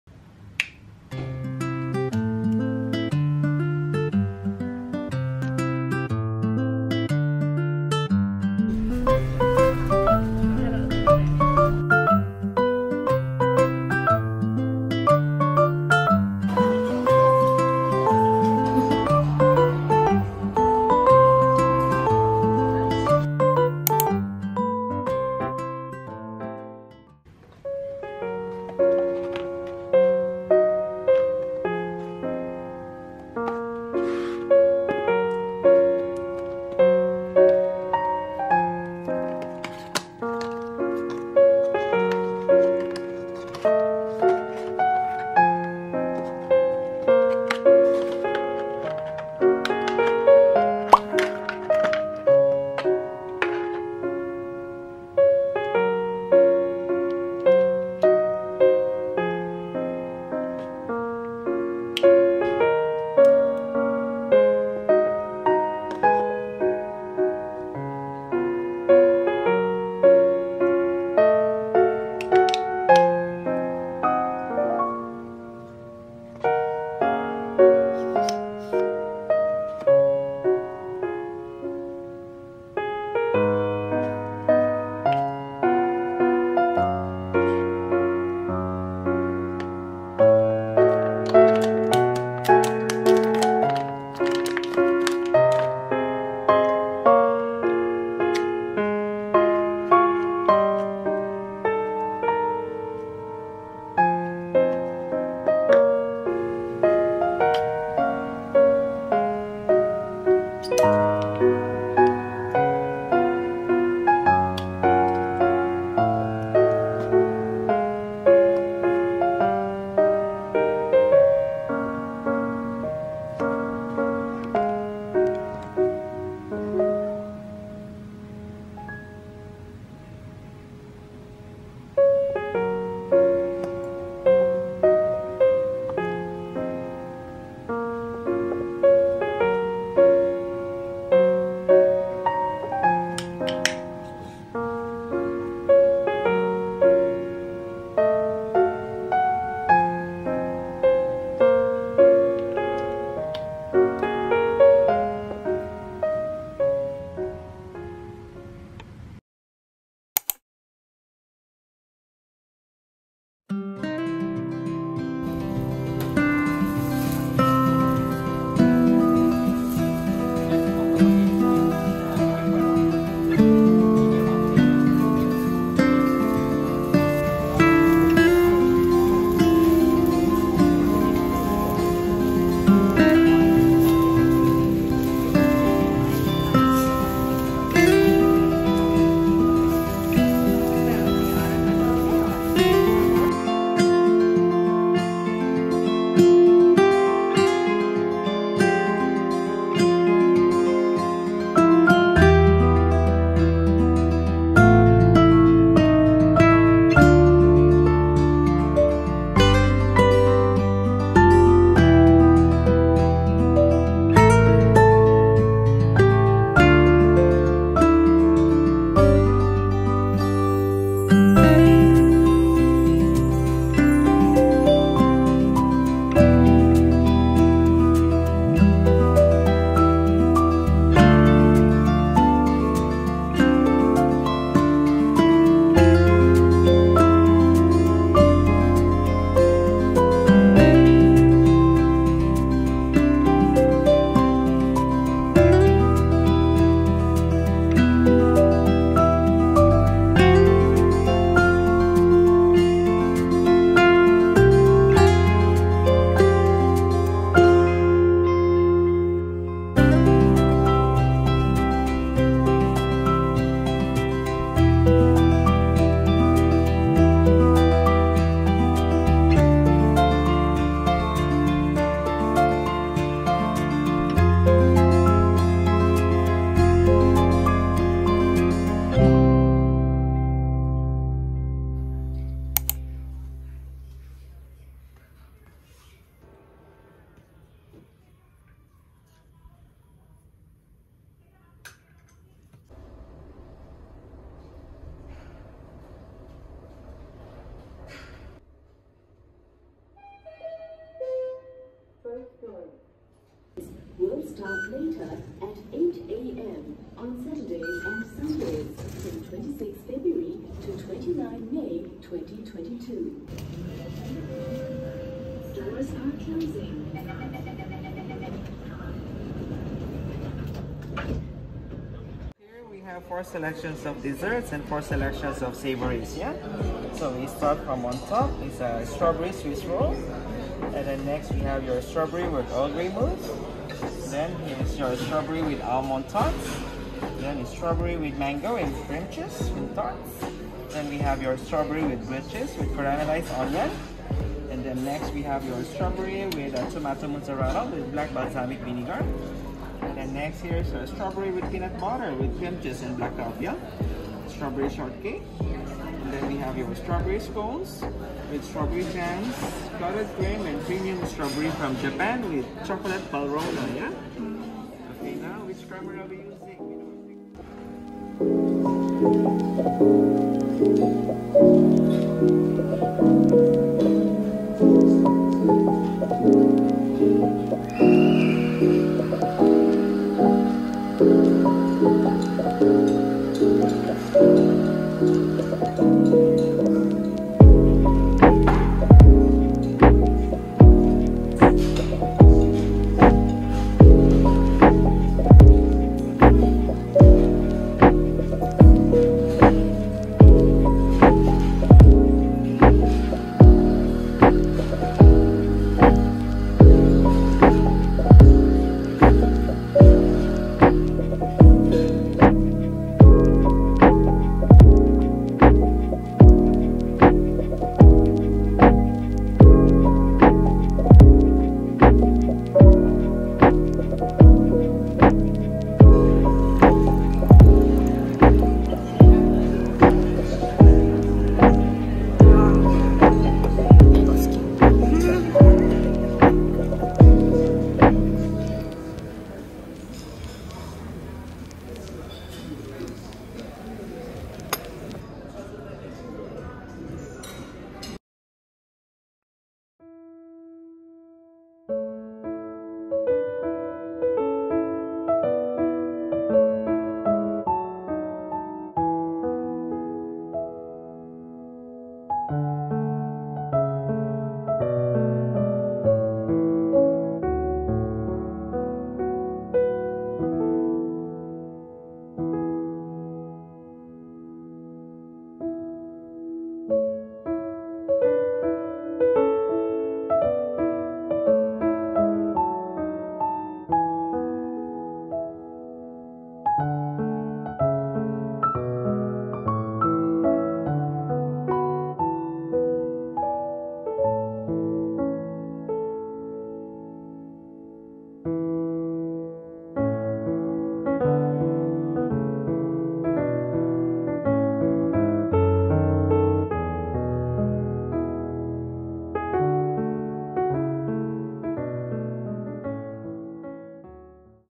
Healing myself | Life in Singapore Vlog | 🍓 Afternoon Tea at Conrad Hotel | Kinu Kappo |Vlog tripod
Subtitle will added soon

Hi,

In this video, I did unboxing of a very affordable tripod which suitable for vlog. I record most of my vlog myself using handphone. So I need reliable tripod.

As I am in transition of working from office, I am trying to be kinder to myself. I went out with my friends for non-seafood Japanese meal at Kinu Kappo.
It is located at Ion Orchard and the main focus of the restaurant is “beef”.

I also went for afternoon tea at Conrad Hotel. It was a luxurious and not too expensive hotel afternoon tea. Strawberry is my favourite fruits so I might be a bit bias. But I love the afternoon tea.

Songs used are

Cheers,
Kath

Link for tripod